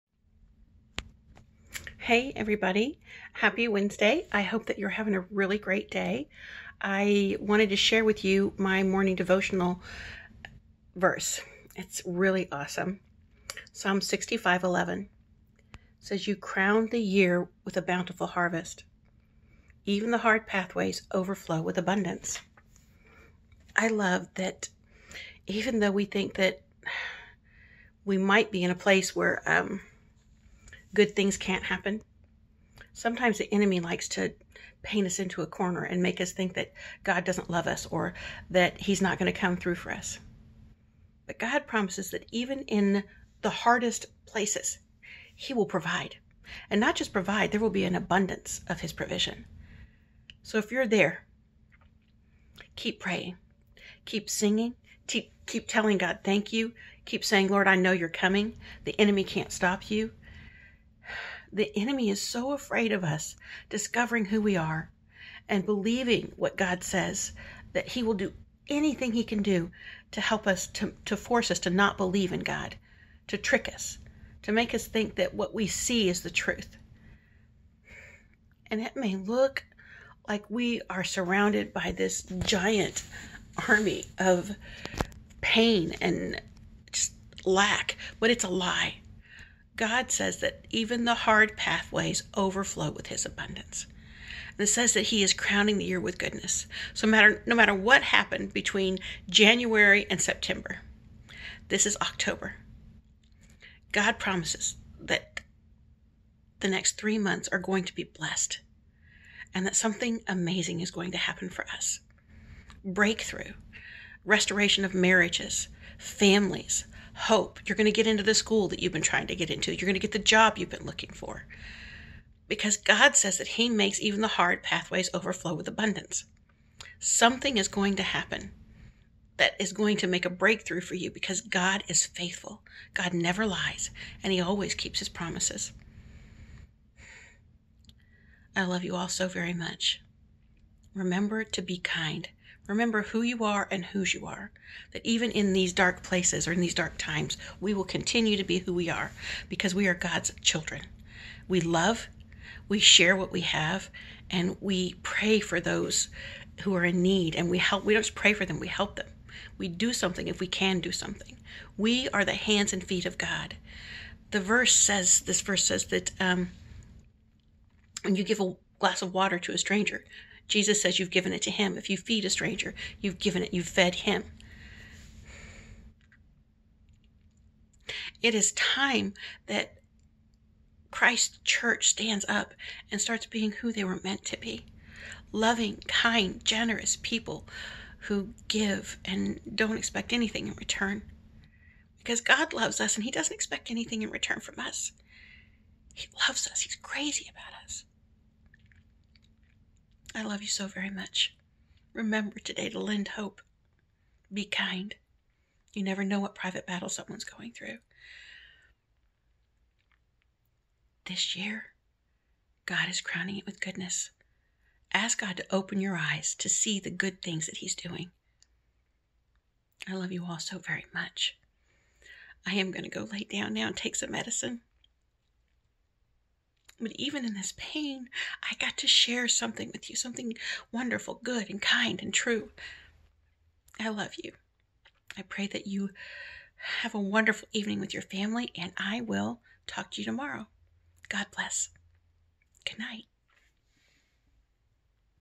God Crowns the Year With Goodness
Even in the wilderness God will bless you!!!
Psalm 65:11 (NLT)
You crown the year with a bountiful harvest; even the hard pathways overflow with abundance.

Psalm 27:13 (NLT)
Yet I am confident I will see the Lord’s goodness while I am here in the land of the living.

Isaiah 43:19 (NLT)
For I am about to do something new. See, I have already begun! Do you not see it?
I will make a pathway through the wilderness. I will create rivers in the dry wasteland.

For I was hungry, and you fed me. I was thirsty, and you gave me a drink. I was a stranger, and you invited me into your home.  I was naked, and you gave me clothing. I was sick, and you cared for me. I was in prison, and you visited me.’

“Then these righteous ones will reply, ‘Lord, when did we ever see you hungry and feed you? Or thirsty and give you something to drink? Or a stranger and show you hospitality? Or naked and give you clothing? When did we ever see you sick or in prison and visit you?’

“And the King will say, ‘I tell you the truth, when you did it to one of the least of these my brothers and sisters, you were doing it to me!’